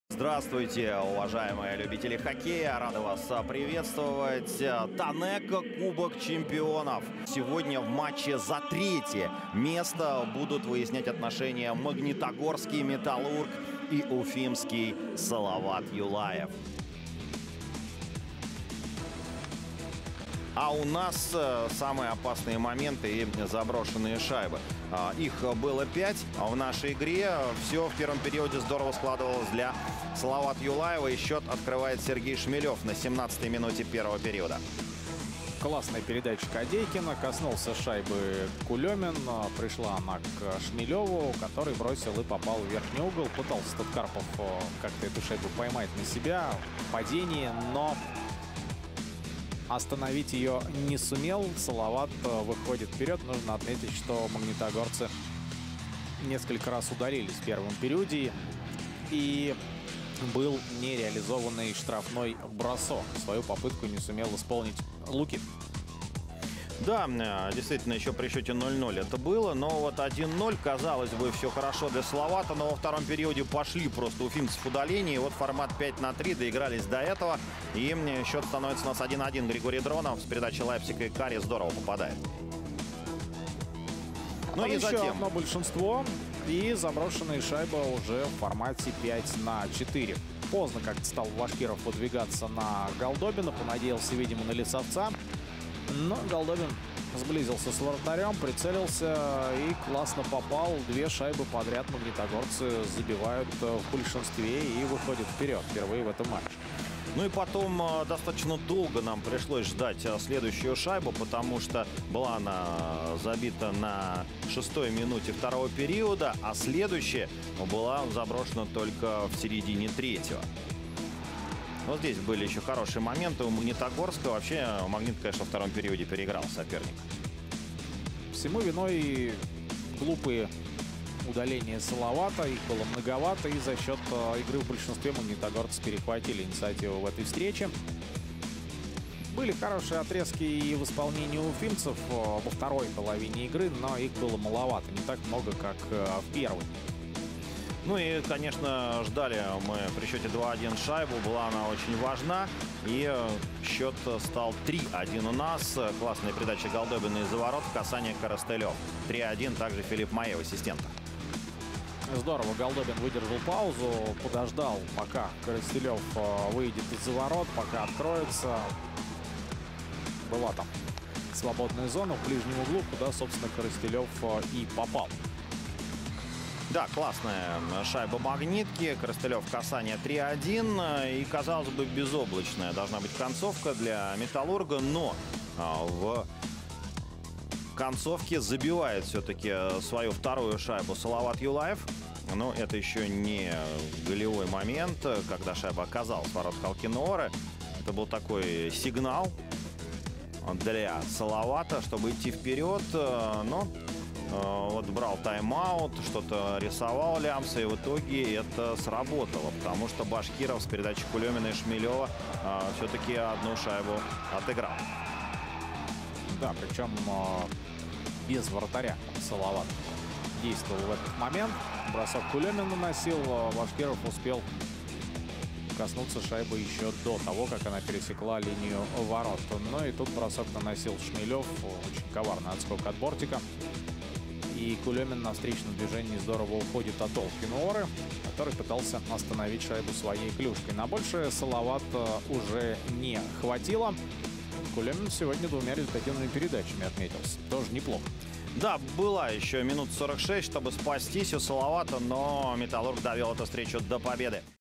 TANECO Cup of Champions. Salavat Yulaev 2 Metallurg Mg 3. 22 August 2021.
0:1 Шмелёв (Кадейкин, Кулёмин) 16:49; 1:1 Дронов (Лайпсик, Карри) 22:15 ГБ; 2:1 Голдобин (Хультстрём, Майе) 25:01 ГБ; 3:1 Коростелёв (Голдобин, Майе, 51:19); 3:2 Башкиров (Кулёмин) 58:14;Вратари: Олкинуора – Шарыченков (00:00-55:42, 57:04-57:10, 58:14-59:01)